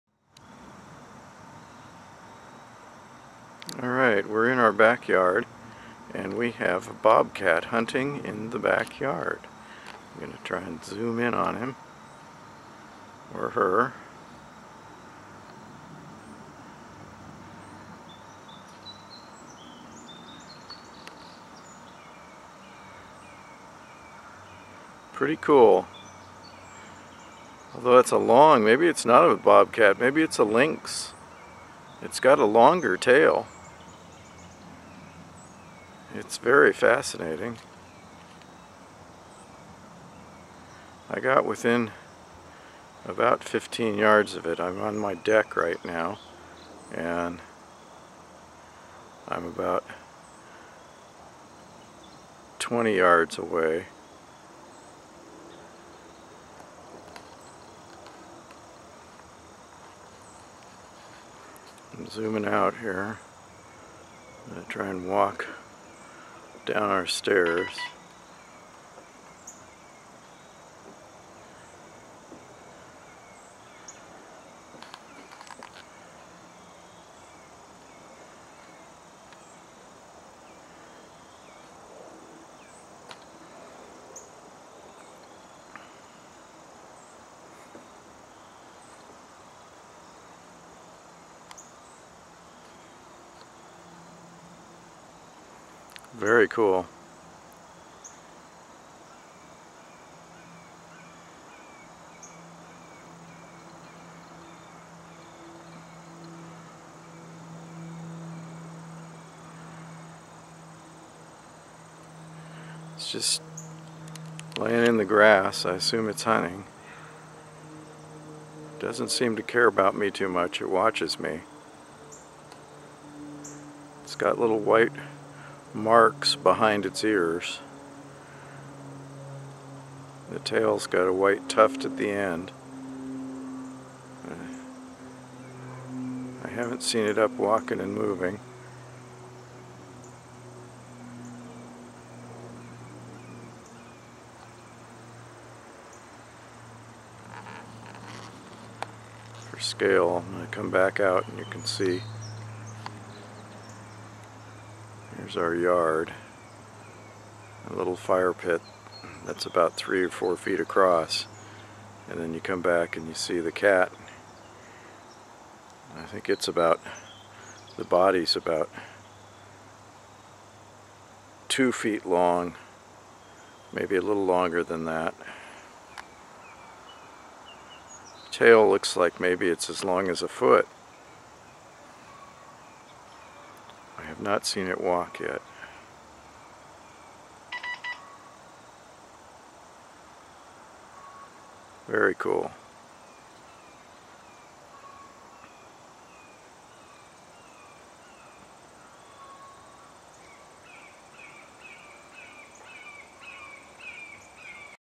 Backyard bobcat
Here's some video of a really nice looking bobcat that was hanging out in our back yard around noon on October 10, 2014. In the video I note how long the tail is (maybe 9 to 12 inches) and speculate that it might be a lynx, but this is definitely a bobcat. Very cool!